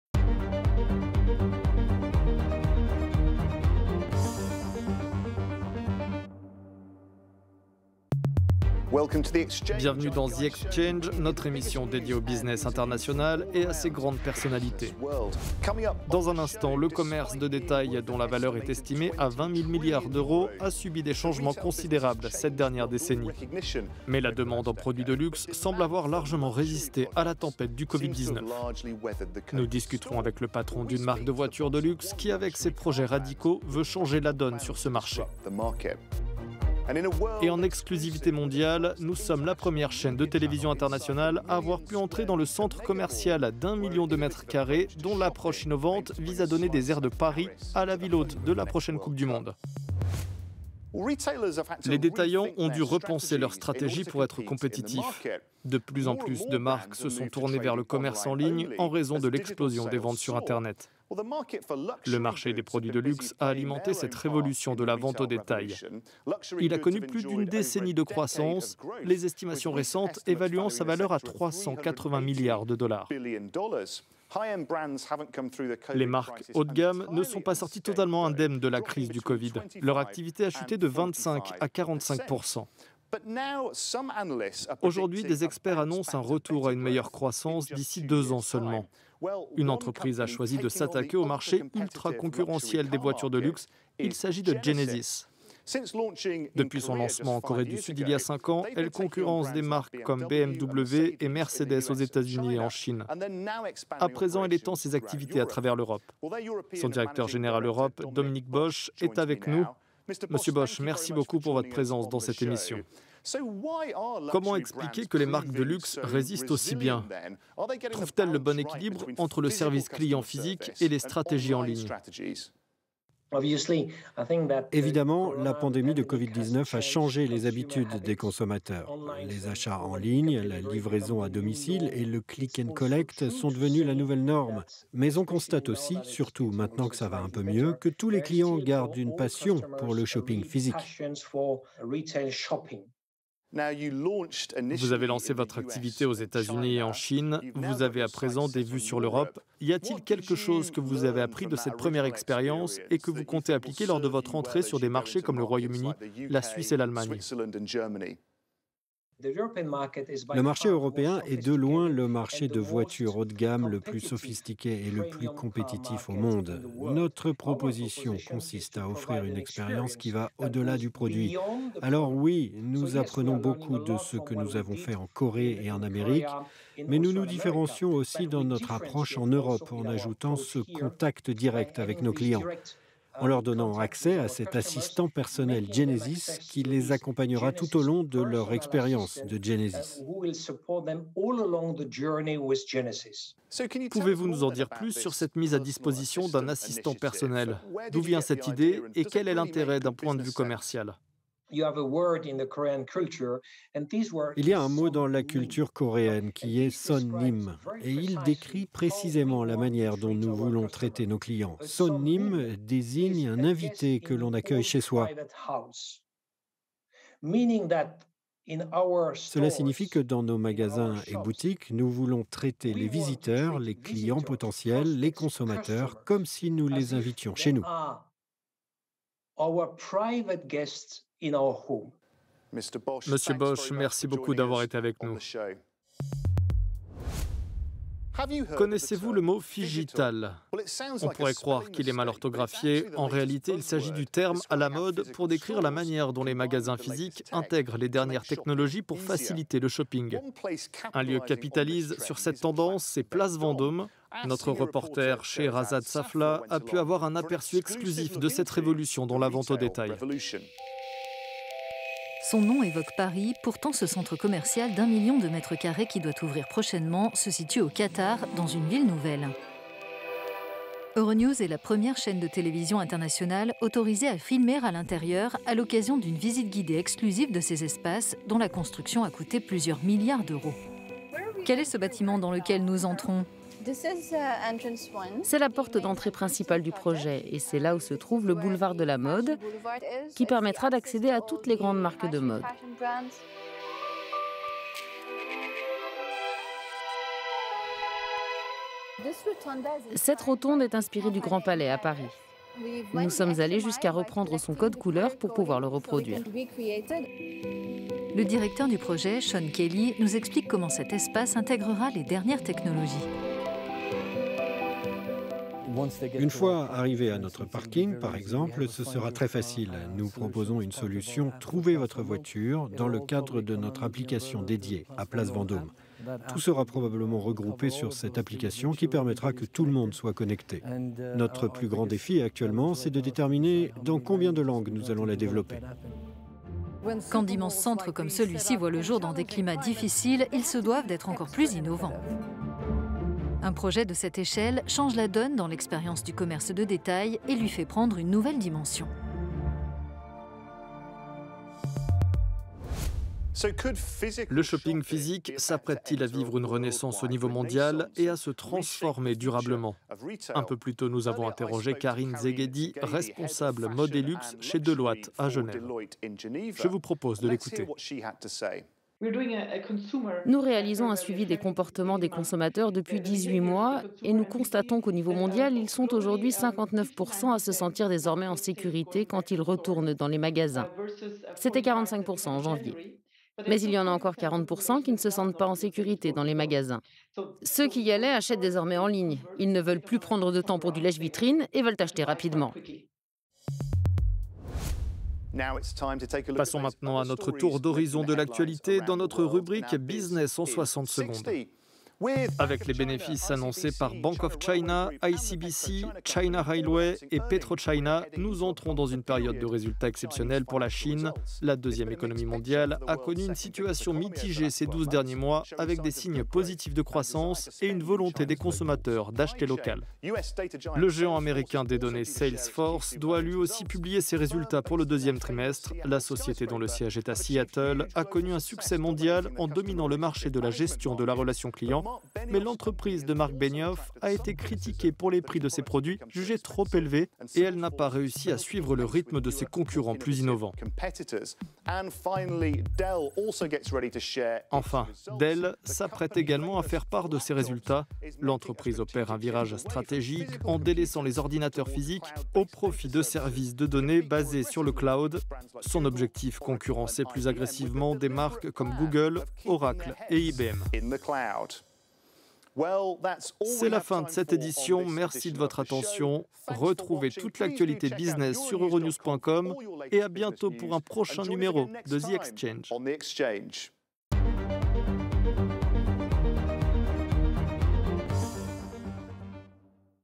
Les marques de luxe alimentent la révolution de la vente au détail
Les achats en ligne ont augmenté de manière spectaculaire du fait de la pandémie. De nombreux magasins ont été contraints de fermer, mais on observe des signes d'une reprise de la demande pour le shopping physique, notamment parmi les marques de luxe.